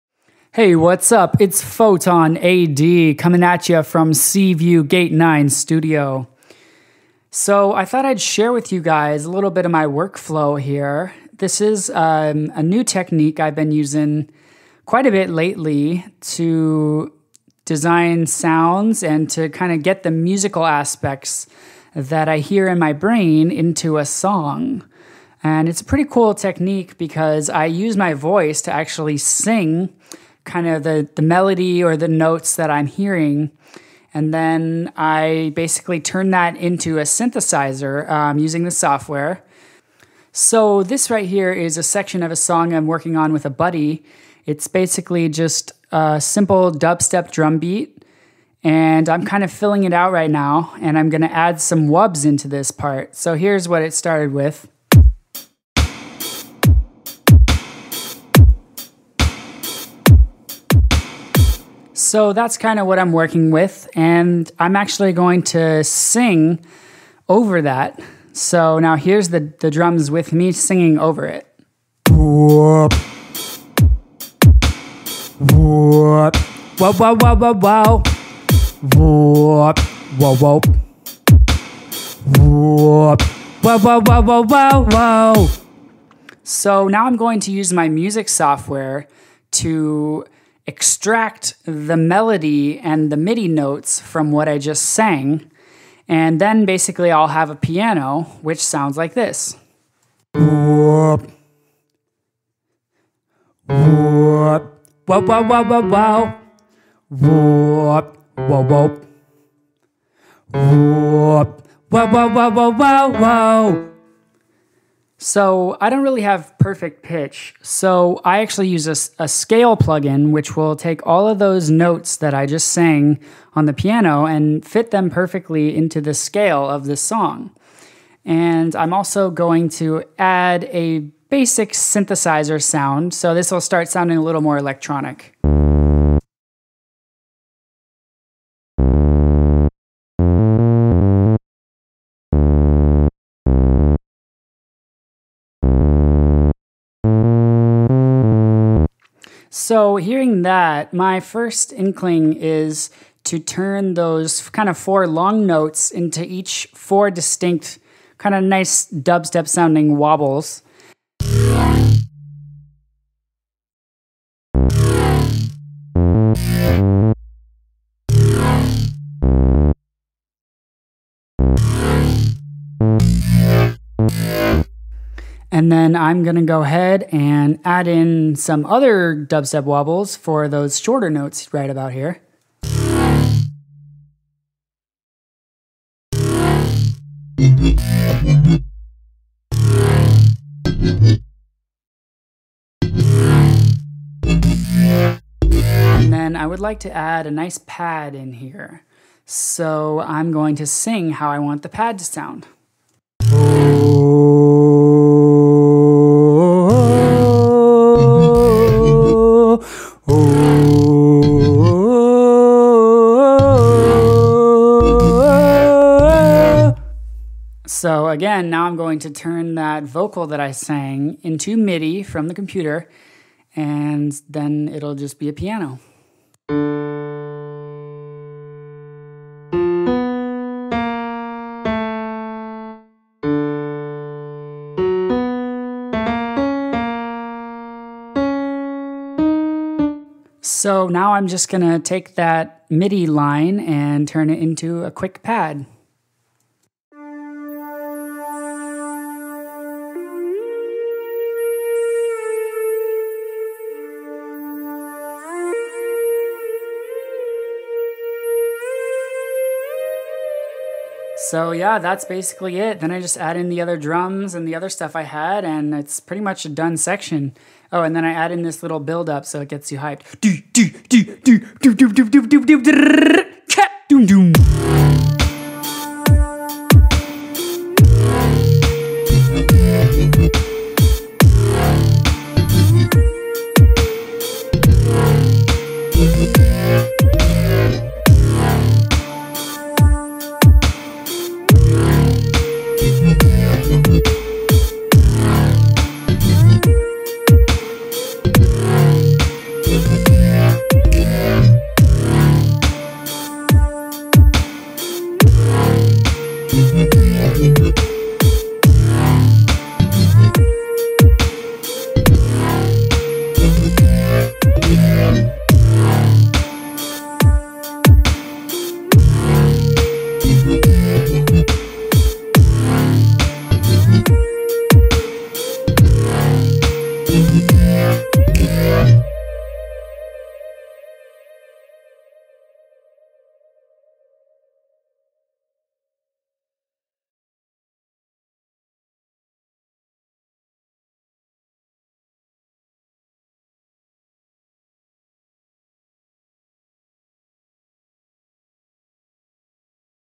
Inside The Studio with Photon A.D. Episode 1- Voice into synth wubs
behind the scenes of a song, Photon A.D. shows you how he turns melodies in his head into full blown dubstep wubs!